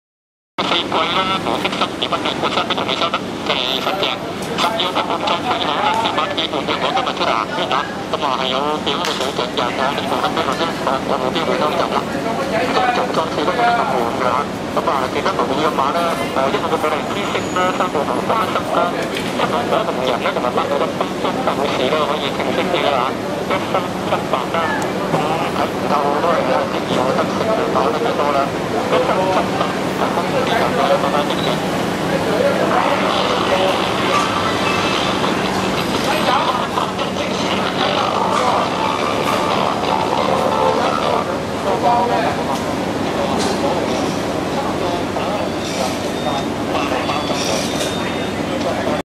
Mongkok soundscape: street sounds in Hong Kong
Parked in a street in Mongkok, a district in Kowloon, Hong Kong, was a cycle used for transporting gas cylinders. For some reason it had a radio in its pannier which was broadcasting to nobody in particular. This and other street sounds were recorded on 29 October 2014.